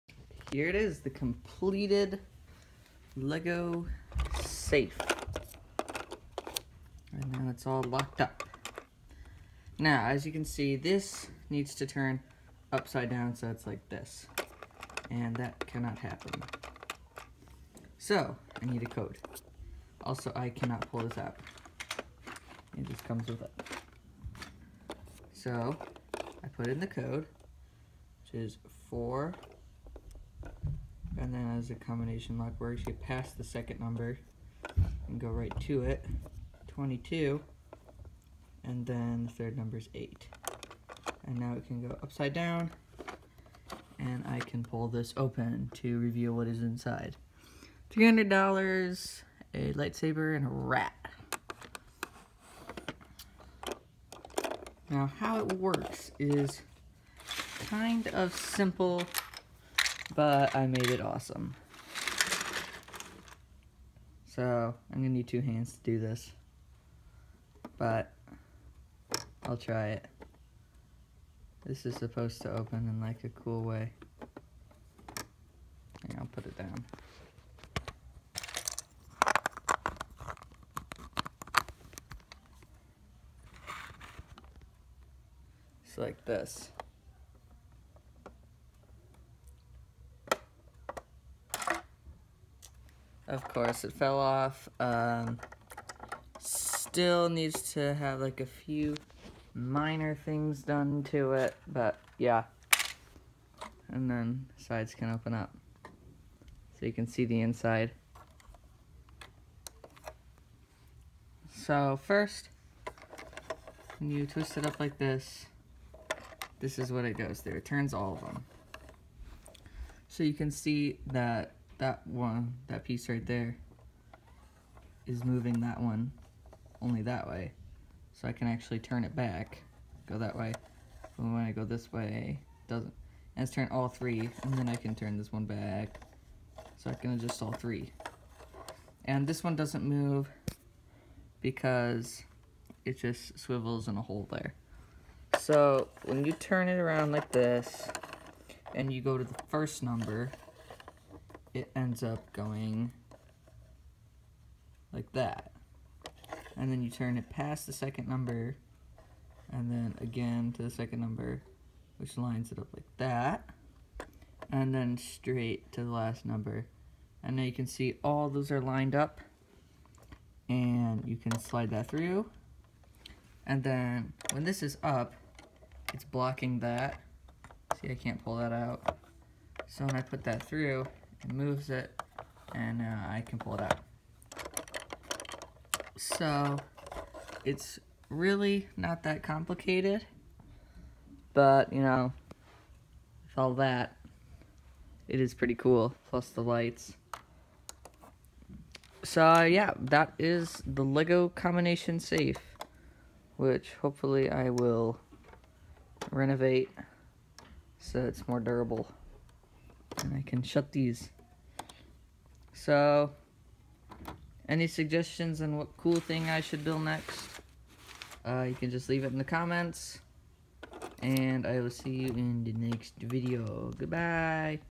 Completed Lego Combination Safe
It’s finished! The opening of the back would’ve been better but it was still cool to see the inside.